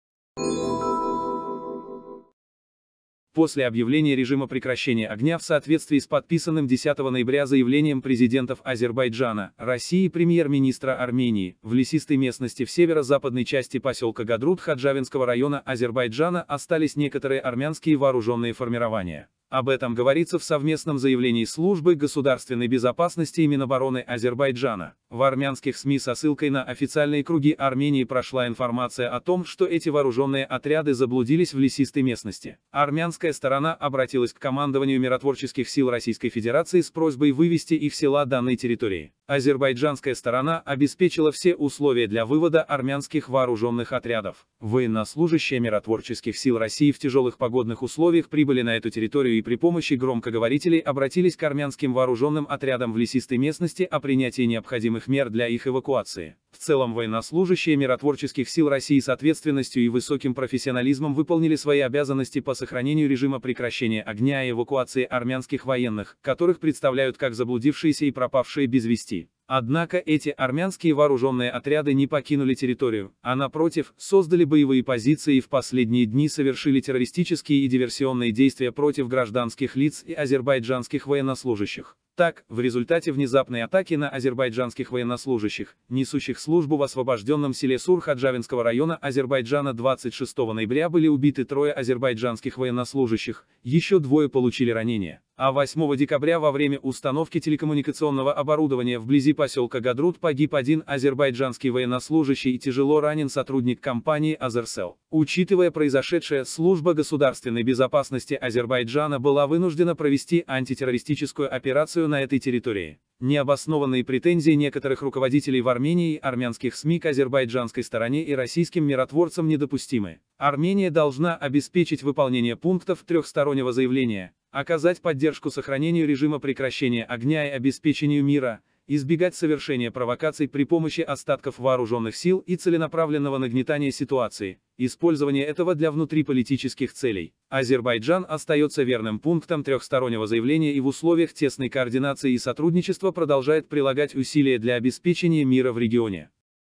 СГБ и Минобороны об армянских солдатах в лесах Гадрута
После объявления режима прекращения огня в соответствии с подписанным 10 ноября заявлением президентов Азербайджана, России и премьер-министра Армении, в лесистой местности в северо-западной части поселка Гадрут Ходжавендского района Азербайджана остались некоторые армянские вооруженные формирования.